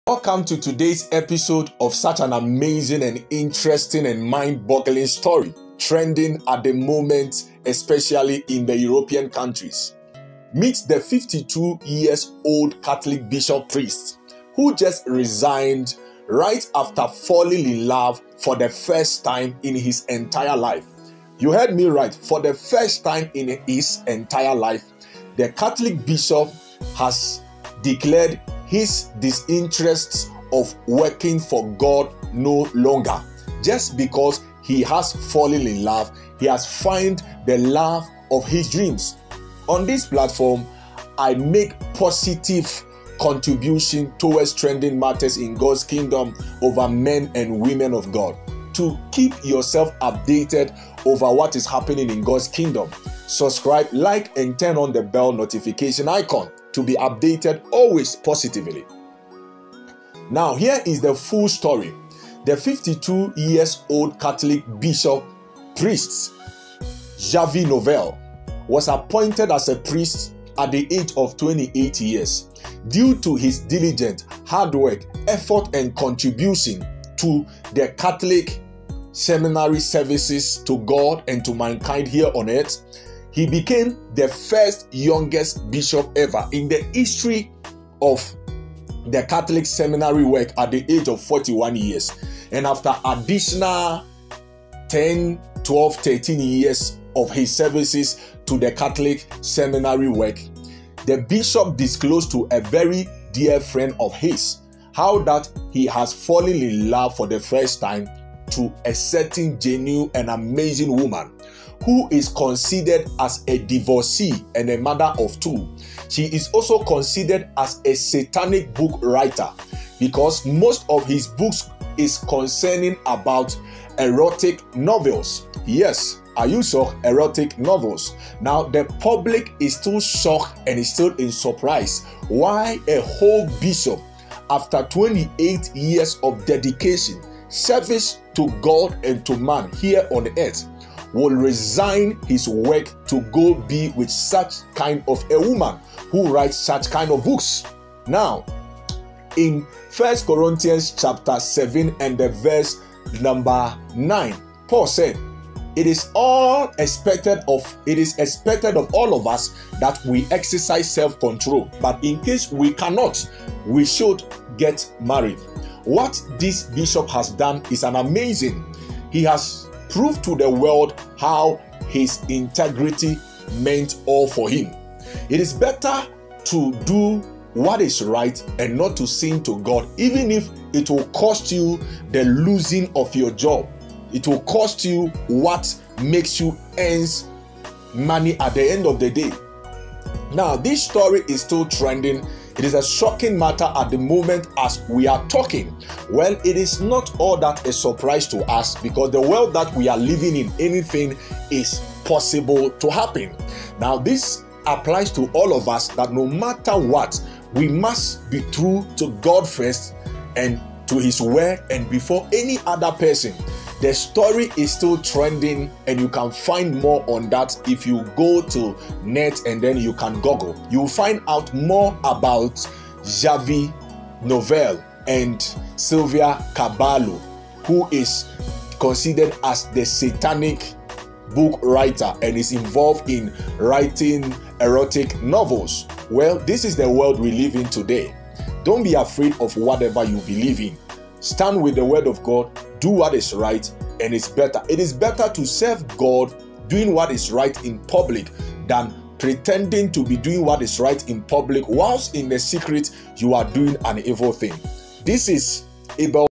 52 Years Catholic Bishop Resign After Falling In Love First Time His Life 😍🥱💯
52 Years Catholic Bishop Resign After Falling In Love First Time Ever In Life - the video consist of story of a 52 Years Catholic Bishop who resign simply because he is found lover of his life, this story happen in Europe Spain, today am telling you a story,so enjoy the video shalom please please I don't own any copy right of the beat under my video for education and and better production video, thanks.